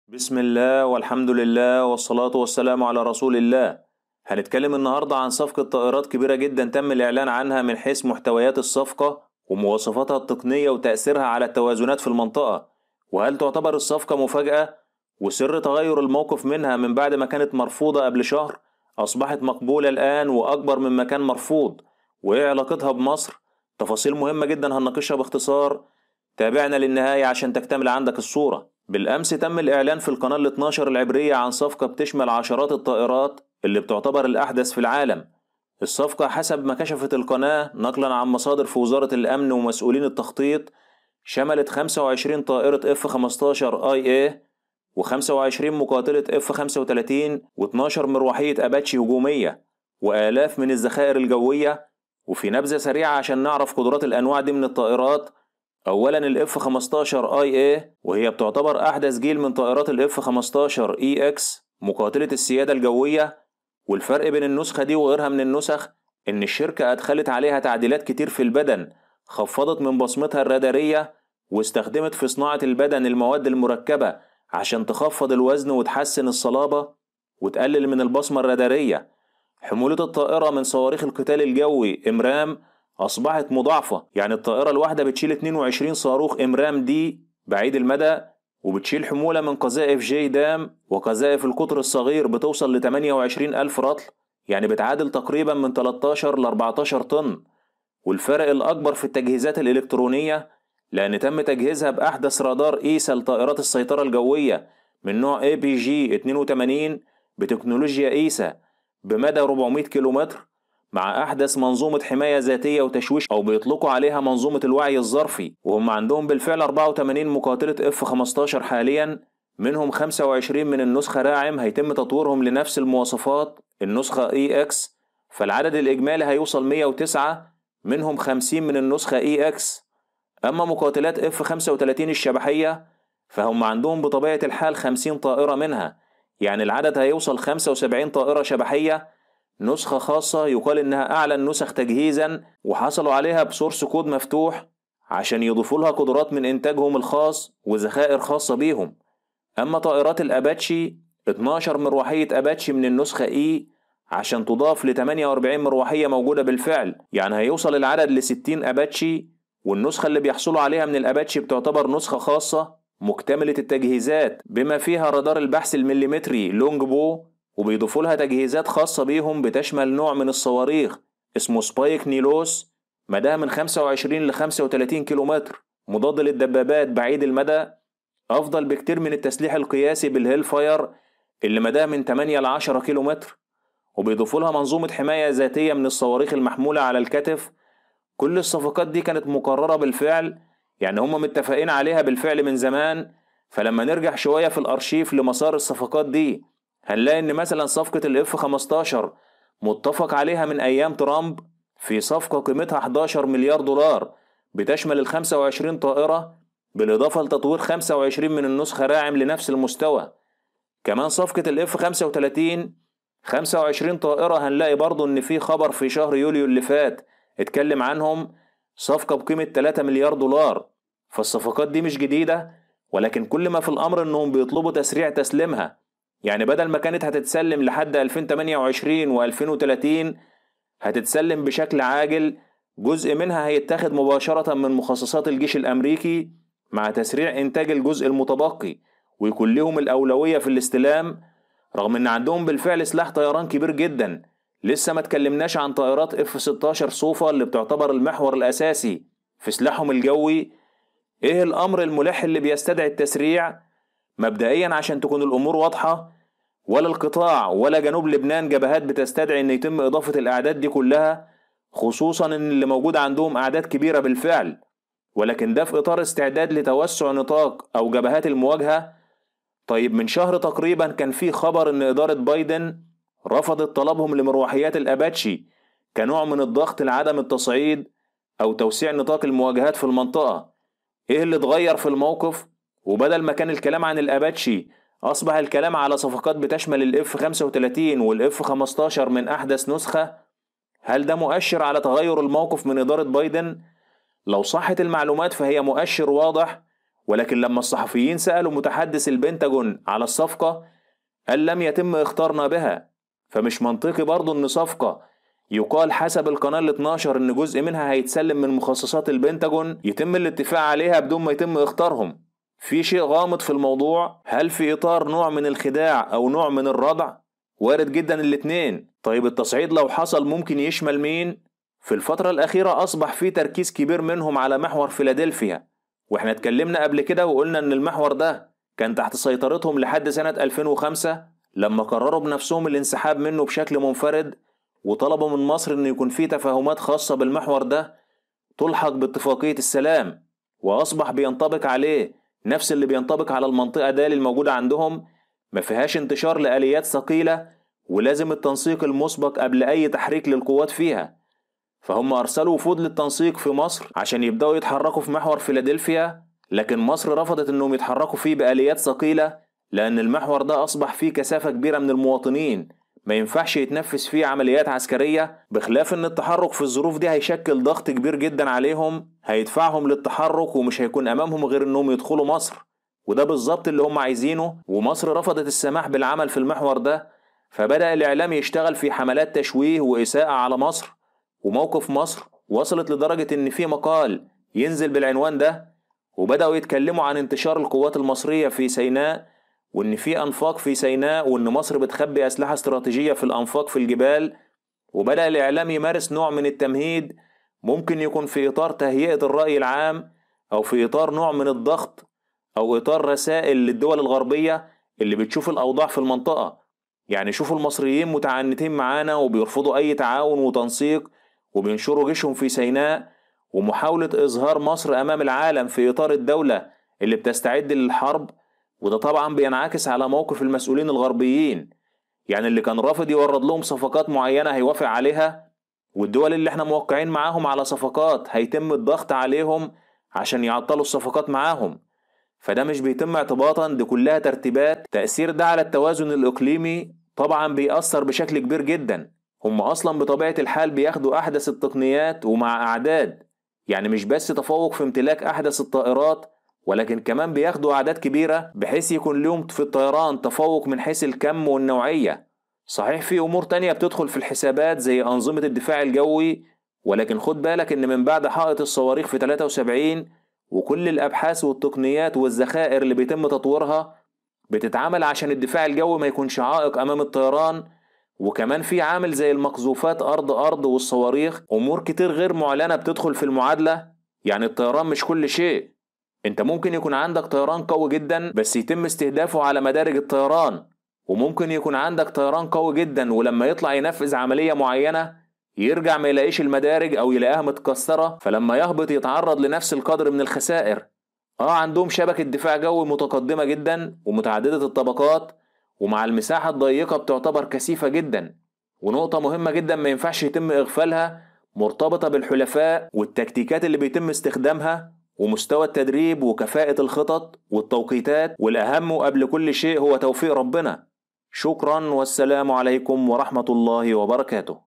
أسرار أكبر صفقة تضم العشرات من طائرات السيادة الجوية و طائرات شبحية F 35 و الاباتشي و علاقتها بمصر
#  محمد_محمود_الثقافة_العسكرية نتناول في هذا الفيديو بالتحليل العلمي و الفني و الخططي , صفقة الطائرات الكبري التي تم اعلانها من القناة ال 12 و ما تضمنته من طارات شملت طائرات السيادة الجوية من نوع F-15
من أحدث جيل و مواصفاتها الفنية , بالاضافة الي صفقة طائرات الجيل الخامس من نوع F-35  و ابرز ما يميزها عن غيرها من النسخ
بالاضافة الي صفقة مروحيات الاباتشي , و ماذا تشكل تلك الصفقة من حيث القدرات الفنية و النوعية بالاضافة الي التفوق العددي و تاثيرها , و خصوصا في ظل التطورات في الشرق_الاوسط و غزة و توتر العلاقات مع مصر بسبب محور_فيلادلفيا و التوازن خصوصا مع